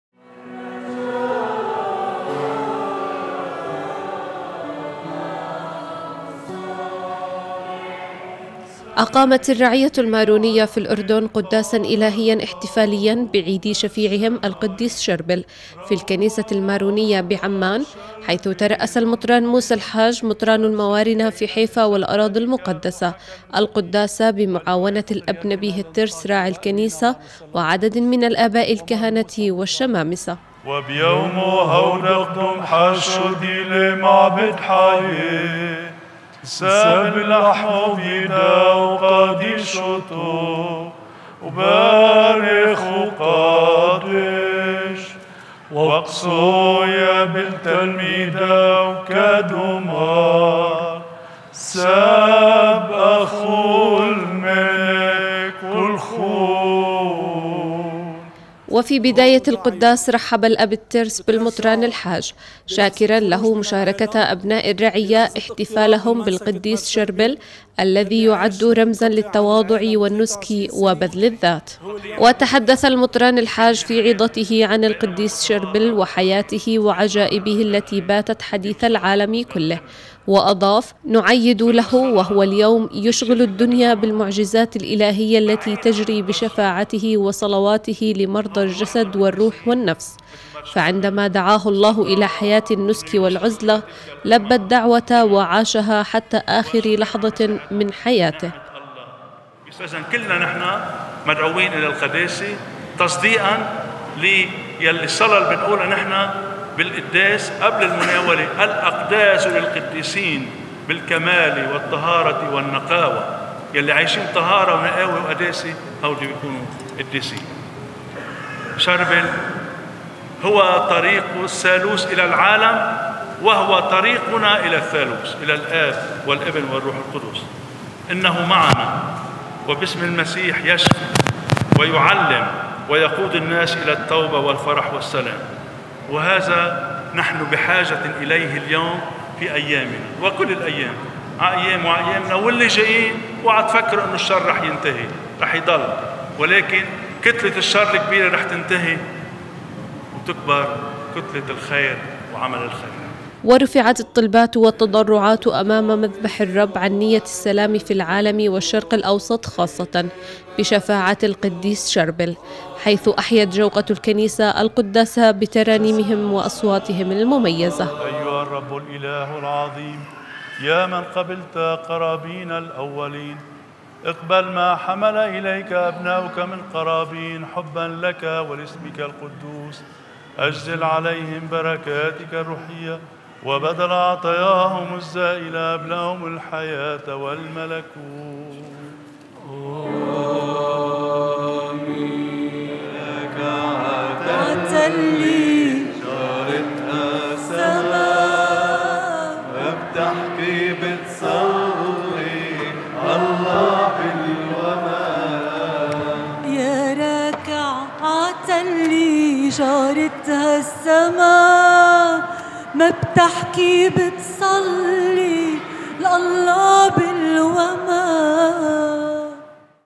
نورسات الأردن : تقرير - الرعية المارونية تحتفل بعيد القديس شربل 2017
اقامت الرعية المارونية في الأردن قداسا الهيا احتفاليا بعيد شفيعهم وأب كنيستهم القديس شربل في الكنيسة المارونية بعمان حيث ترأس المطران موسى الحاج مطران الموارنة في حيفا والأراضي المقدسة القداس بمعاونة الأب نبيه الترس راعي الكنيسة وعدد من الآباء الكهنة والشمامسة.
وفي بداية القداس رحب الأب الترس بالمطران الحاج شاكرا له مشاركة ابناء الرعية احتفالهم بالقديس شربل الذي يعد رمزا للتواضع والنسك وبذل الذات
وتحدث المطران الحاج في عظته عن القديس شربل وحياته وعجائبه التي باتت حديث العالم كله واضاف: نعيّد له وهو اليوم يشغل الدنيا بالمعجزات الإلهية التي تجري بشفاعته وصلواته لمرضى الجسد والروح والنفس. فعندما دعاه الله إلى حياة النسك والعزلة، لبّى الدعوة وعاشها حتى آخر لحظة من حياته
ورفعت الطلبات والتضرعات امام مذبح الرب عن نية السلام في العالم والشرق الاوسط خاصة بشفاعة القديس شربل حيث احيت جوقة الكنيسة القداس بترانيمهم واصواتهم المميزة.